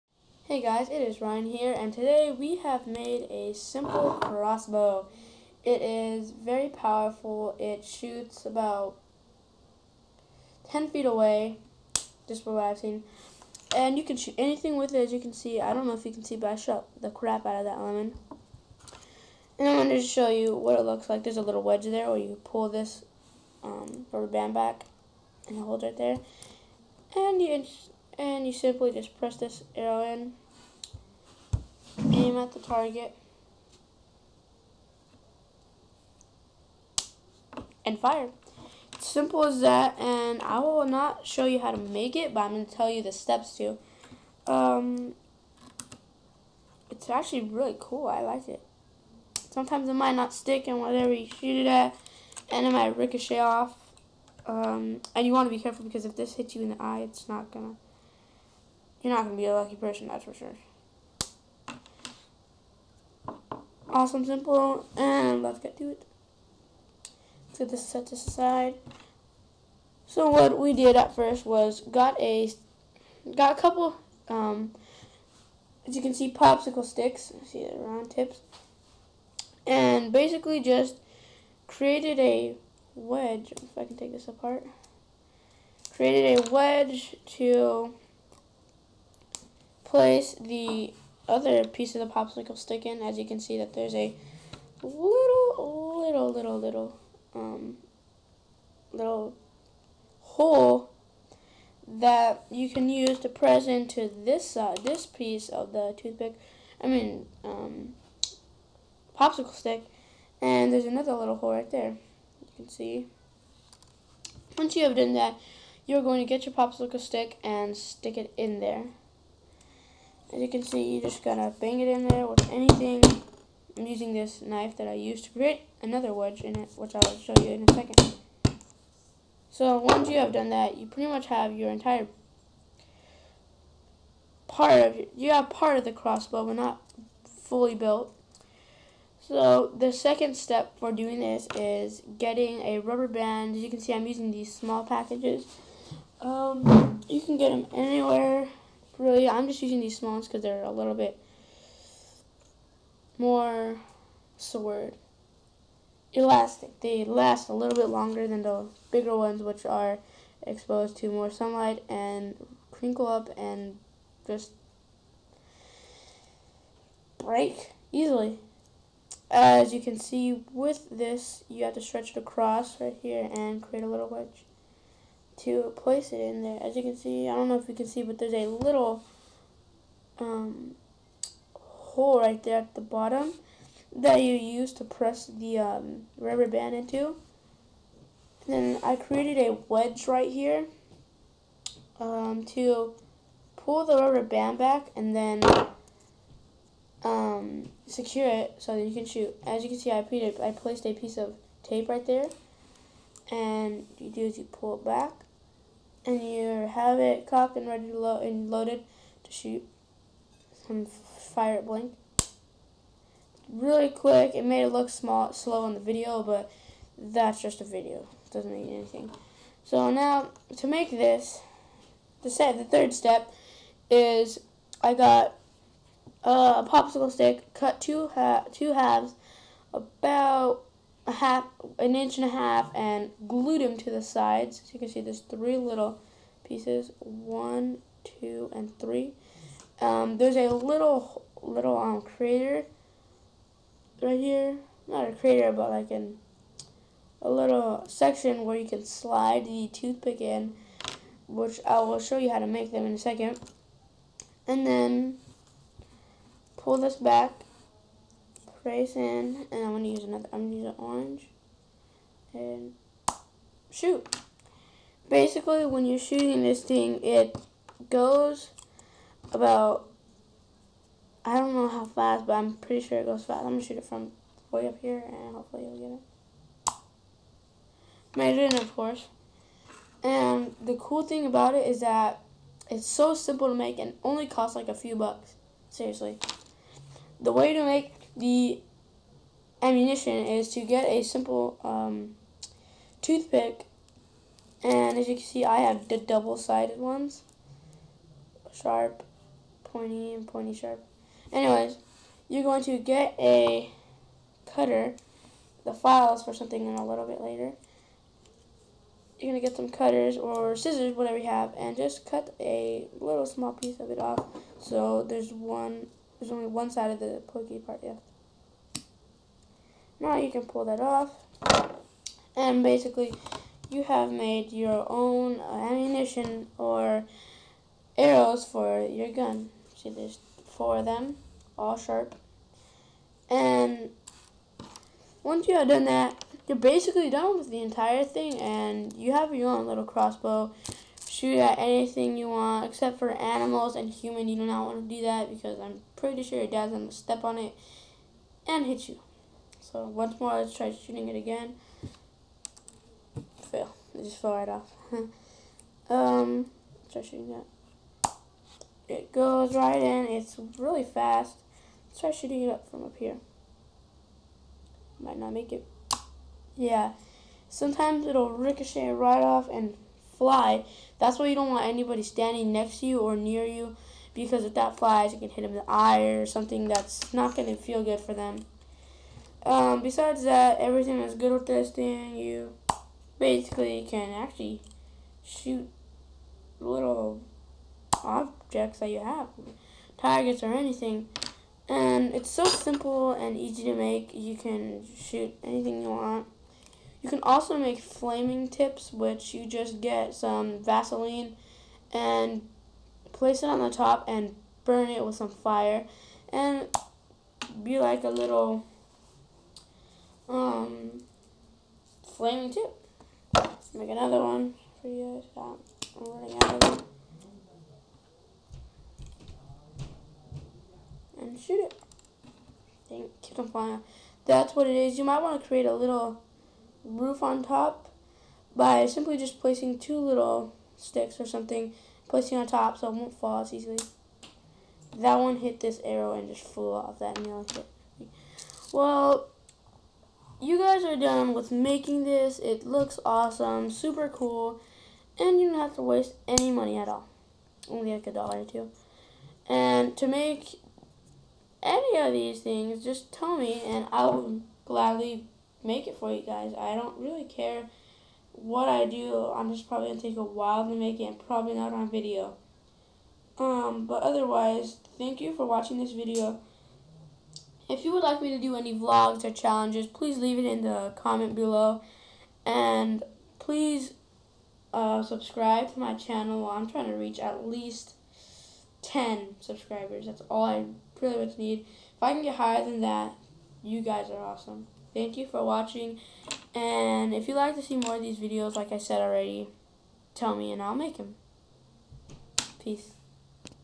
How to make a simple but powerful crossbow !!!!!
Do not aim this weapon at any one! It is very cool but very dangerous .